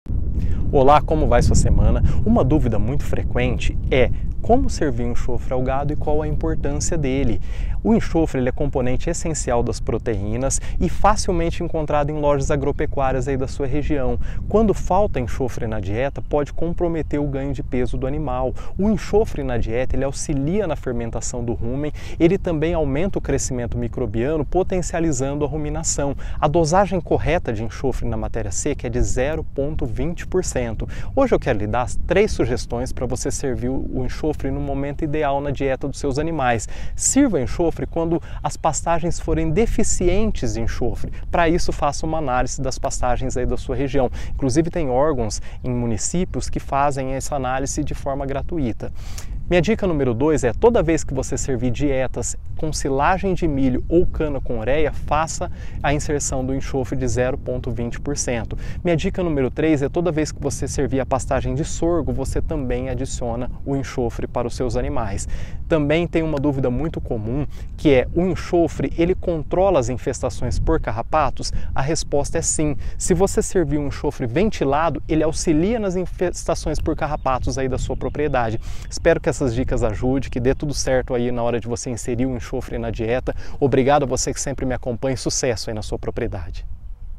COMO SERVIR ENXOFRE PARA O GADO?
Saiba Mais: Uma das dúvidas mais frequentes recebidas pela Boi Saúde é como oferecer o enxofre para o gado. Por que utilizar, dosagem certa, quando e como, são algumas delas. Nesta dica, vamos te oferecer um conteúdo direcionado para que você possa promover a melhor nutrição possível ao seu rebanho.